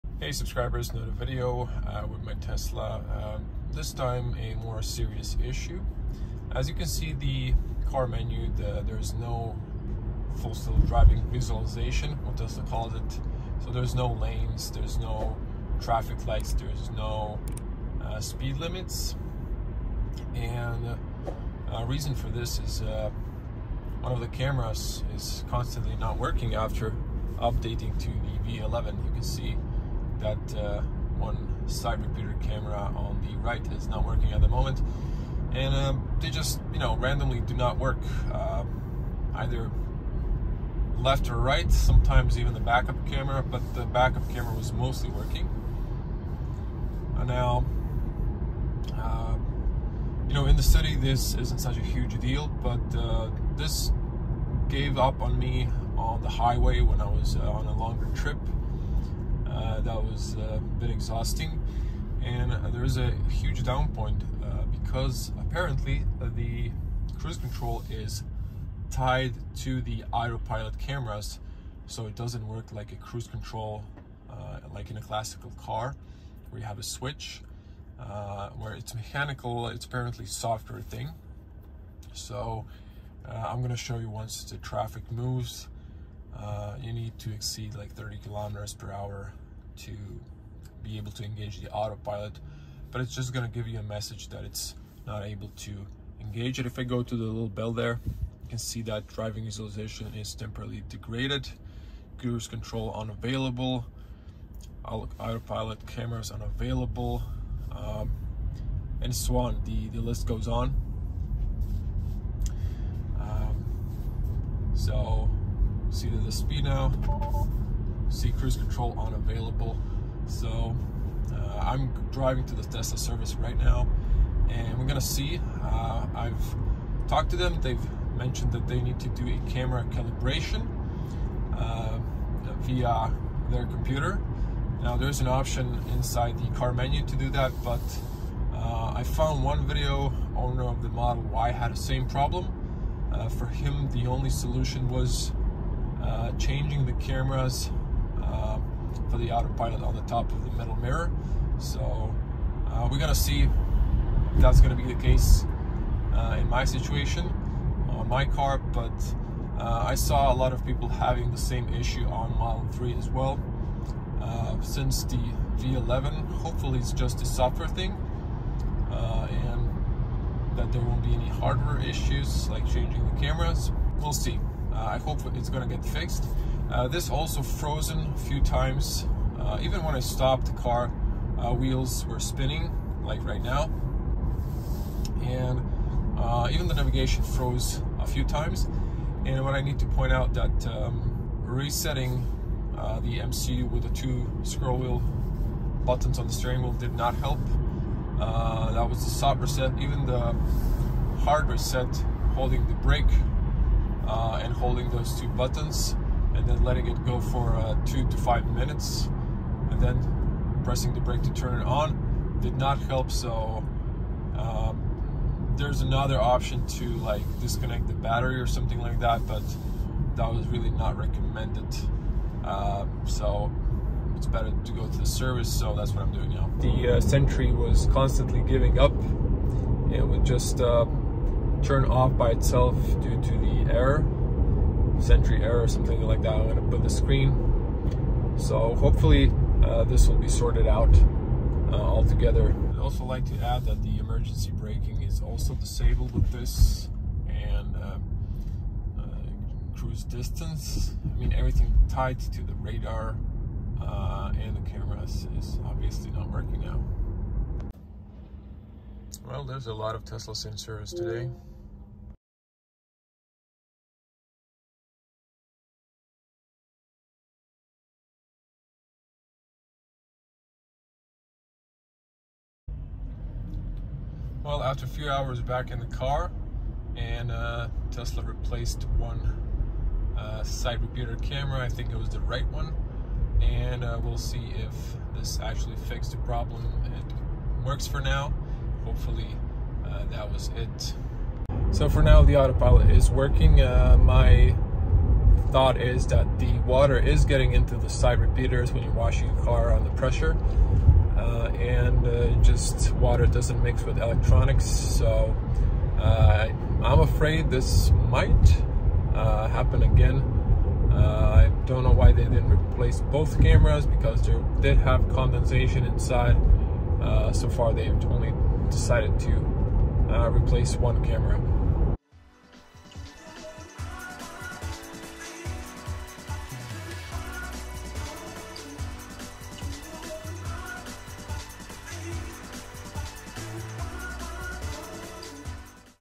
Tesla Autopilot not working (NO; FSD Visualization, Cruise Control, Sentry, ...)
Tesla Autopilot not working (NO FSD Visualization, NO Cruise Control, NO Sentry)

If you experienced in your Tesla that Autopilot and FSD visualization has stopped working and one of the cameras is not working. You should visit the nearest Tesla Service.

Unfortunately, it appears that cruise control, radar distance, and emergency braking are all tied to Autopilot cameras if one stops working, all systems are unavailable and you are stuck with basic car functions. Obviously, your Sentry will turn off as well.

Tesla error codes:
UI_a114 (Driving visualization temporarily degraded)
DI_a175 (Cruise control unavailable)
APP_w305 (Autopilot cameras unavailable)

Soft and hard reset won't help. Hardware (camera) replacement is necessary. In my case, the side repeater camera was replaced and solved the issue.

UPDATE: Both side repeater cameras were replaced including the harness as it corroded due to water getting in the side repeater cameras! This is an obviously needed recall that Tesla is ignoring and will be costly repair outside of warranty!